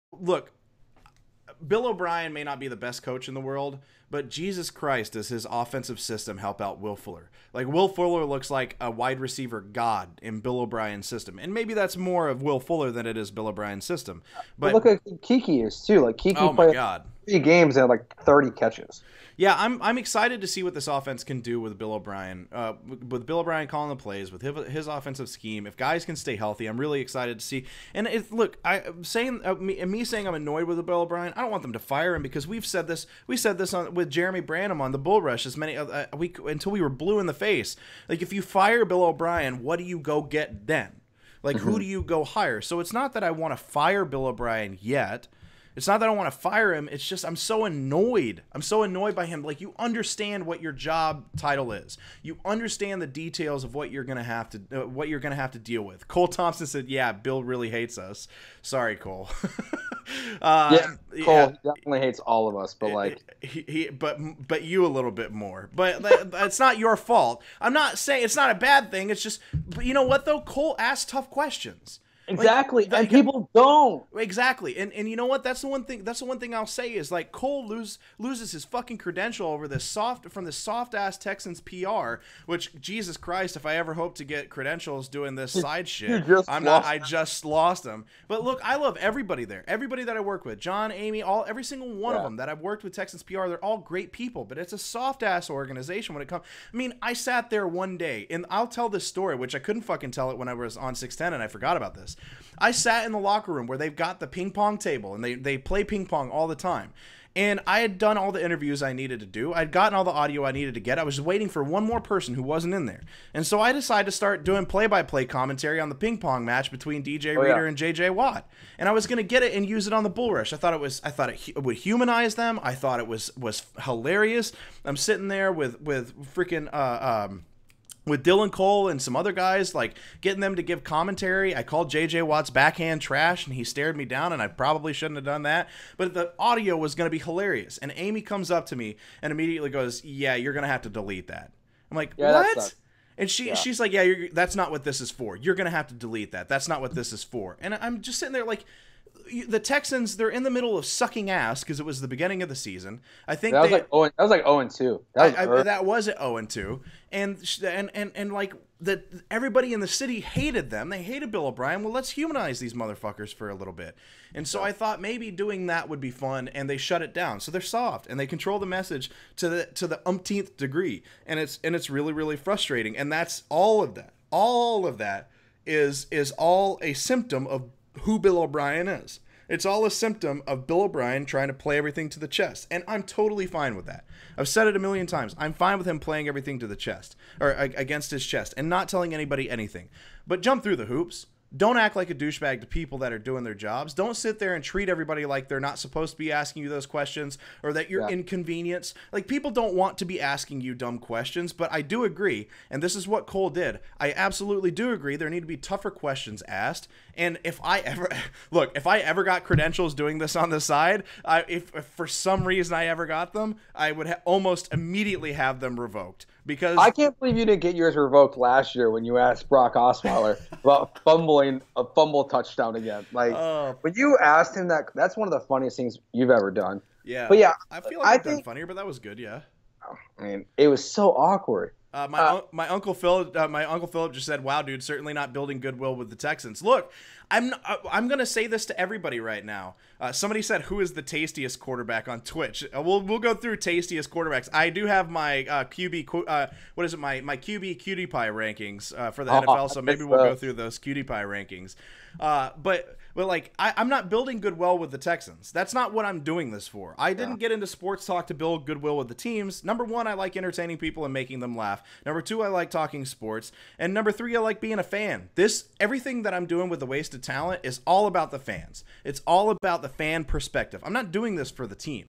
Wasted Clips w/ Josh Beard | Bill O'Brien And The Houston Texans Organization Are Soft
The way Bill O'Brien treats fans and media for asking questions he doesn't like is ridiculous. He and the Texans are a soft as butter organization. Joe George and I discuss it!